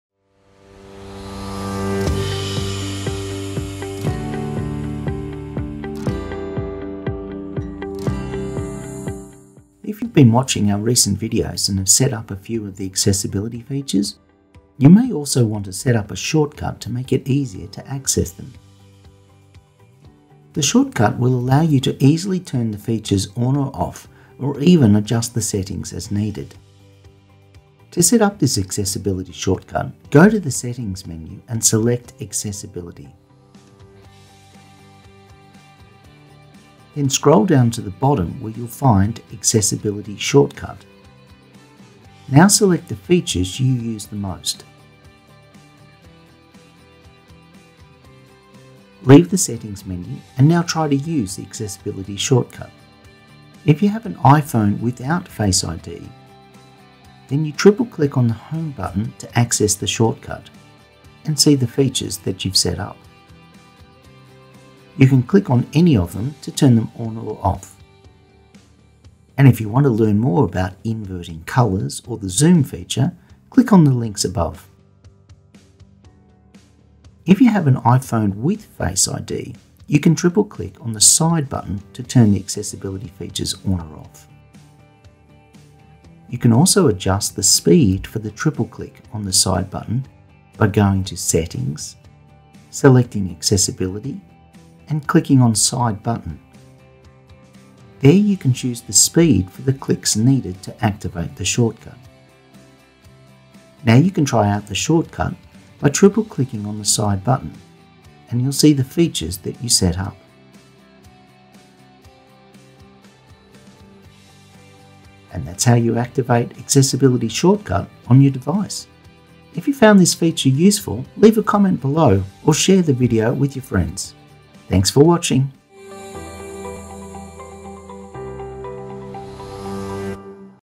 Accessibility Shortcut - iPhone accessibility feature
Video Transcript:

If you've been watching our
recent videos and have set up a few of the accessibility features,
you may also want to set up a shortcut
to make it easier to access them.
The shortcut will allow you to easily turn the features on or off
or even adjust the settings as needed
to set up this accessibility shortcut.
Go to the settings menu and select accessibility.
Then scroll down to the bottom where you'll find accessibility shortcut.
Now select the features you use the most.
Leave the settings menu and now try to use the accessibility shortcut.
If you have an iPhone without face I.D.,
then you triple click on the home button to access the shortcut
and see the features that you've set up.
You can click on any of them to turn them on or off.
And if you want to learn more about inverting colors
or the zoom feature, click on the links above.
If you have an iPhone with face I.D., you can triple click on the side
button to turn the accessibility features on or off.
You can also adjust the speed for the triple click on the side button.
By going to settings, selecting accessibility
and clicking on side button
there you can choose the speed
for the clicks needed to activate the shortcut.
Now you can try out the shortcut by triple clicking on the side button
and you'll see the features that you set up.
And that's how you activate accessibility shortcut on your device.
If you found this feature